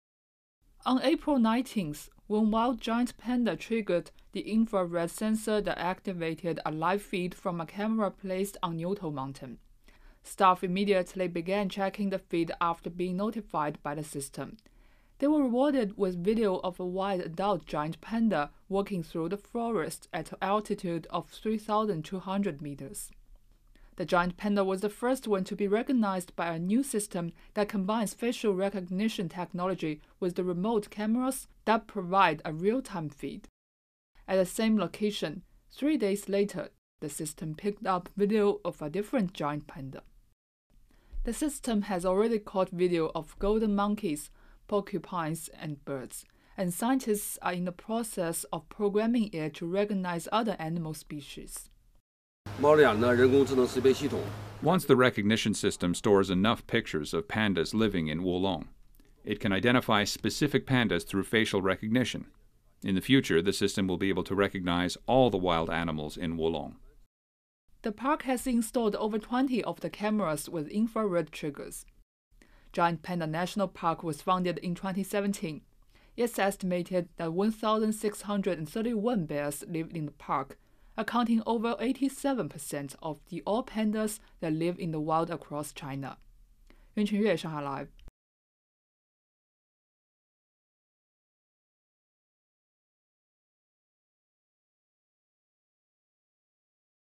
🐼 Real-time facial recognition APP - This time for wild pandas!
The Wolong area of SW China's Sichuan Giant Panda National Park has released video of a giant panda in the wild. The bear triggered a new surveillance system that transmits high definition video in real time and uses facial recognition software.